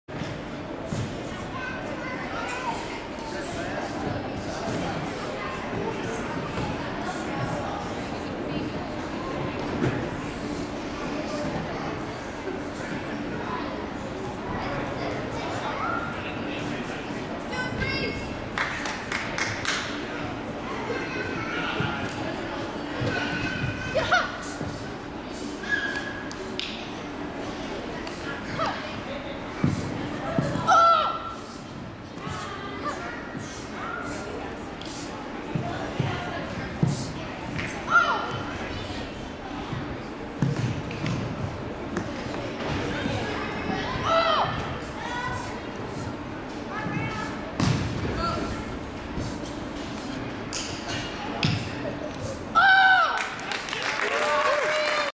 Briann Ginter Weapons Form Derrico Tournament Fall 2012
Karate America Wisconsin Rapids Special Forces Member Briann Ginter Weapons Form Derrico Tournament Fall 2012